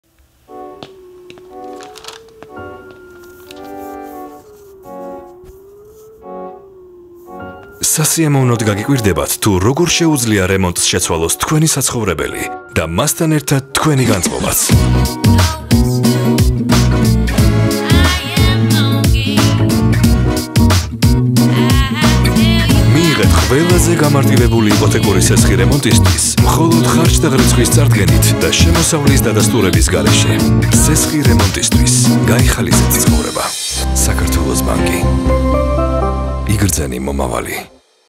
სესხი რემონტისთვის
საქართველოს ბანკის განახლებული სესხი რემონტისთვის, საგაზაფხულო სესხების კოლექციის ფარგლებში